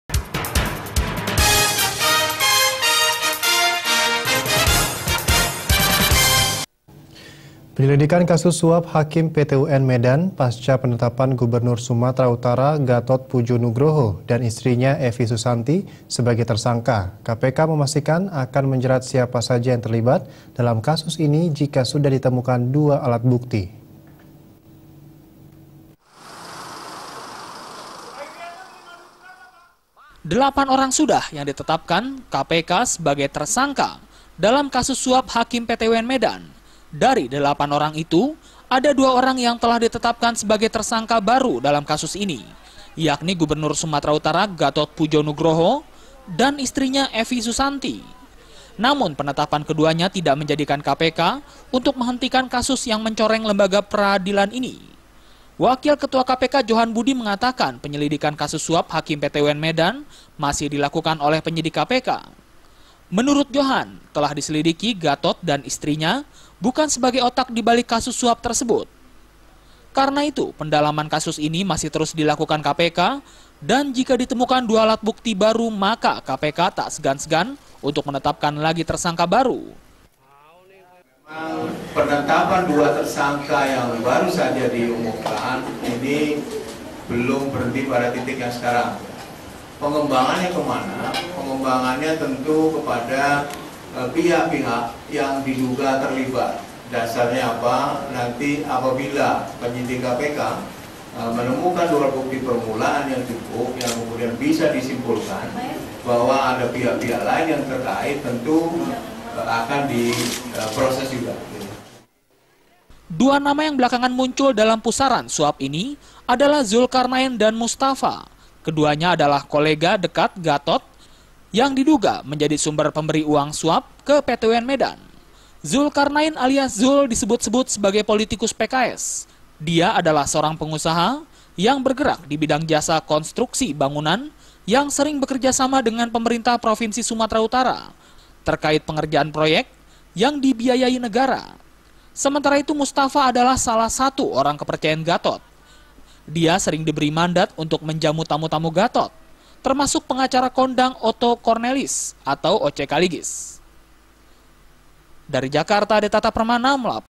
KASUS HAKIM PTUN TAK BERHENTI DI GATOT
PENYELIDIKAN KASUS SUAP HAKIM PTUN MEDAN PASCA PENETAPAN GUBERNUR SUMUT, GATOT PUJO NGROHO DAN ISTRINYA EVI SUSANTI SEBAGAI TERSANGKA. KPK MEMASTIKAN AKAN MENJERAT SIAPA SAJA YANG TERLIBAT DALAM KASUS INI, JIKA SUDAH DITEMUKAN 2 ALAT BUKTI.